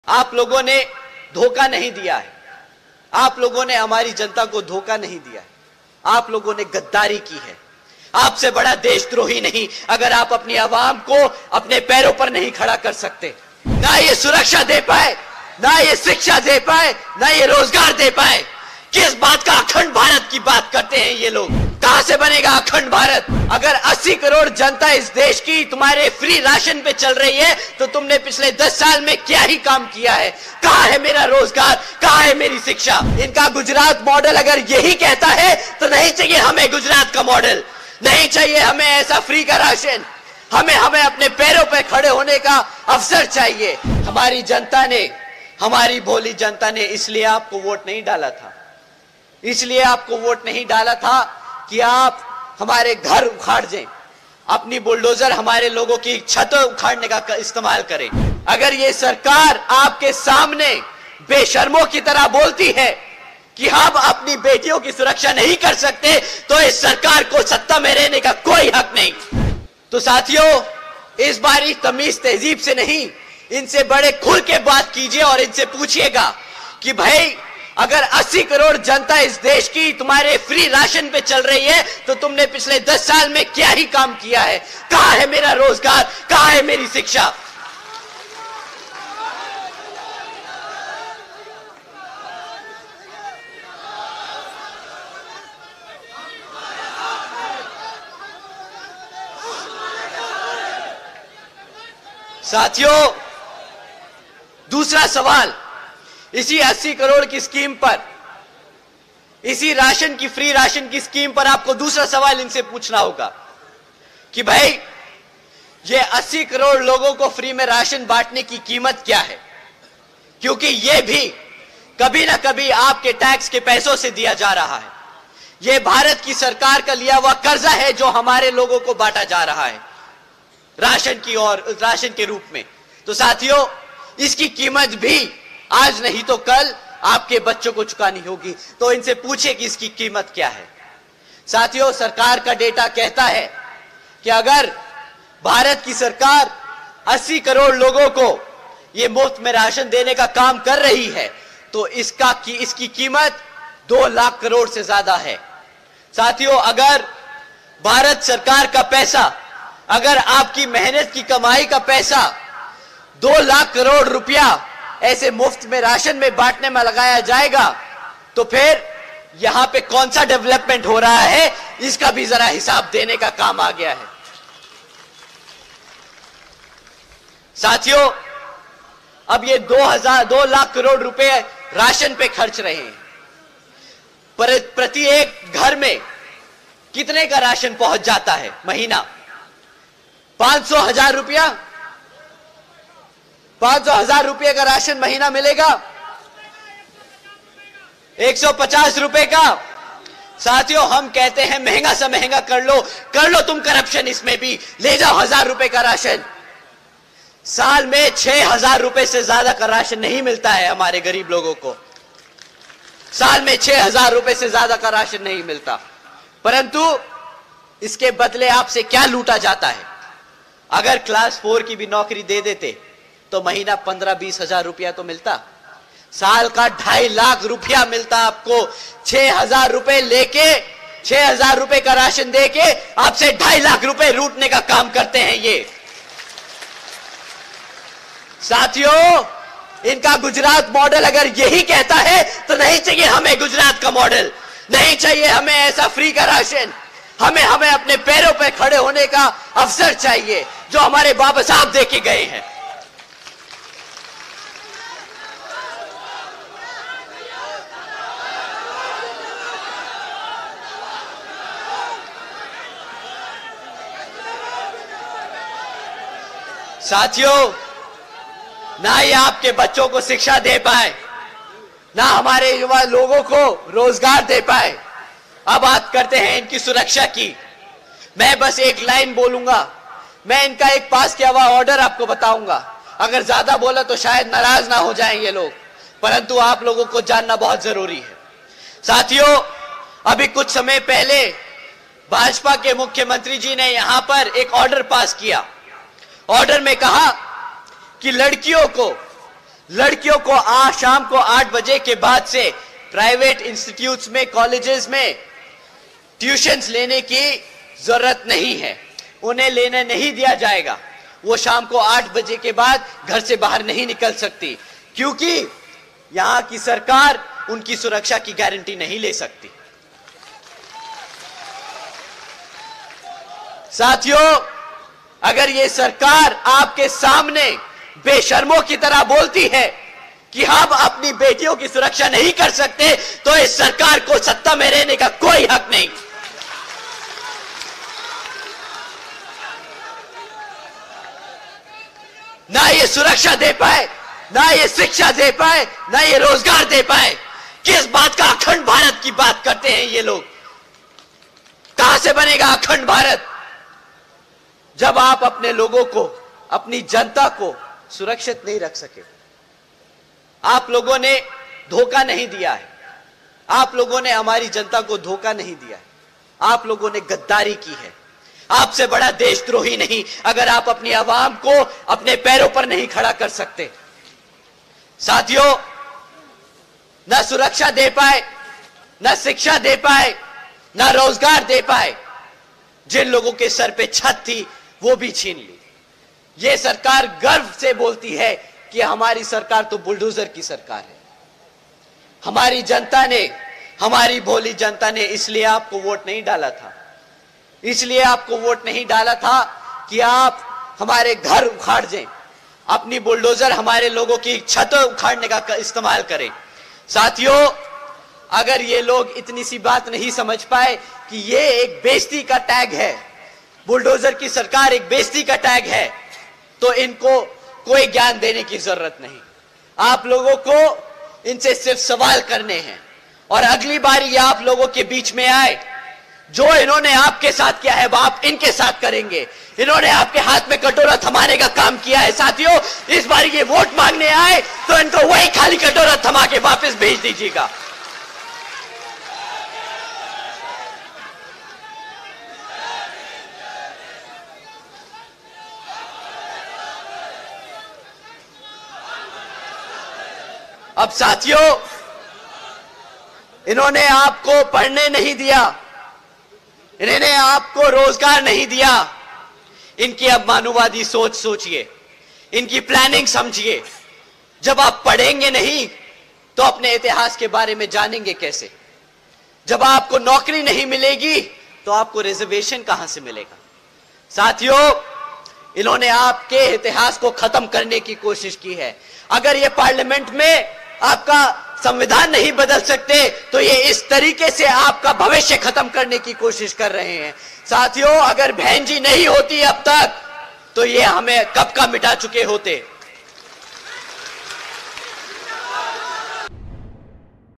PM Modi, CM Yogi पर खूब बरसे BSP Chief Mayawati के भतीजे Akash Anand| Loksabha Election| Paper Leak
Loksabha Election 2024: BSP national Co-ordinator and nephew of former CM Mayawati Akash Anand attacked PM Modi and UP CM Yogi Adityanath. In the Loksabha Election rally at Bareilly, Akash raised the issues of unemployment, women's security, education and jobs. Akash Anand Slams UP government for bulldozer action and Paper leak cases. Watch video to know more.